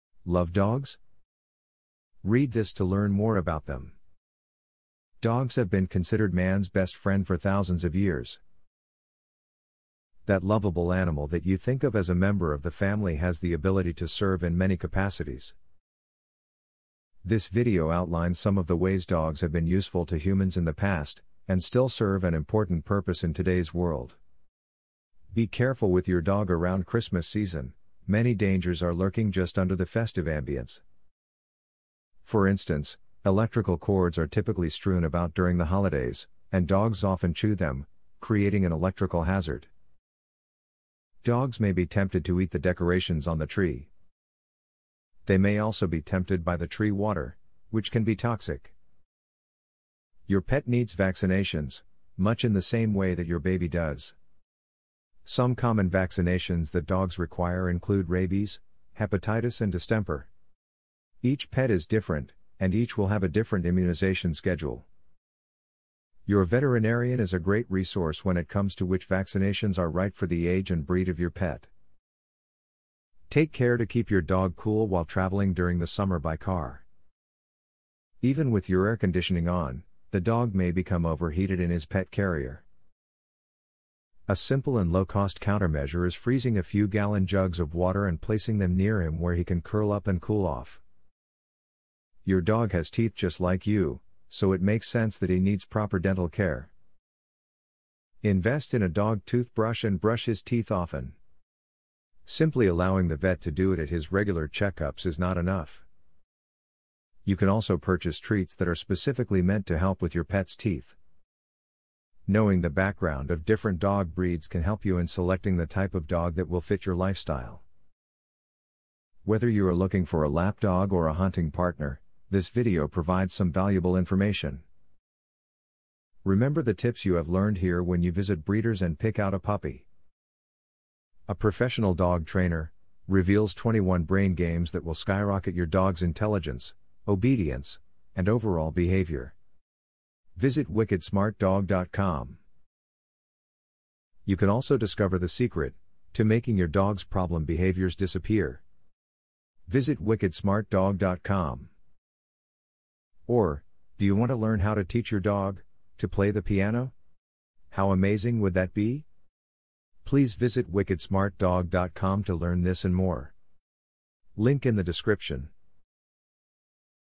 Love Dogs?
Love Dogs? Read This To Learn More About Them. Dogs have been considered mans' best friend for thousands of years. That lovable animal that you think of as a member of the family has the ability to serve in many capacities. This video outlines some of the ways dogs have been useful to humans in the past, and still serve and important purpose in today's world.